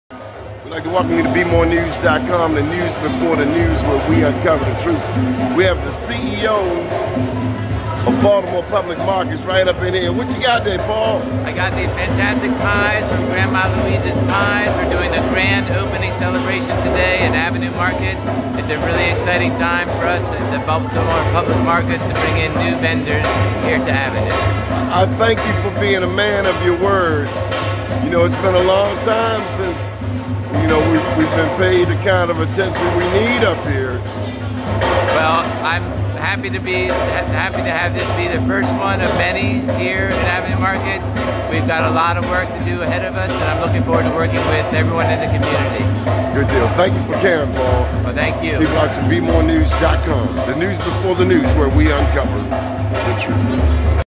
Paul Ruppert, CEO of Baltimore Public Markets, at Grand Opening of Mama Louise's Pies
Paul Ruppert, CEO of Baltimore Public Markets, at Grand Opening of Mama Louise's Pies at the Avenue Market on Historic Pennsylvania Avenue.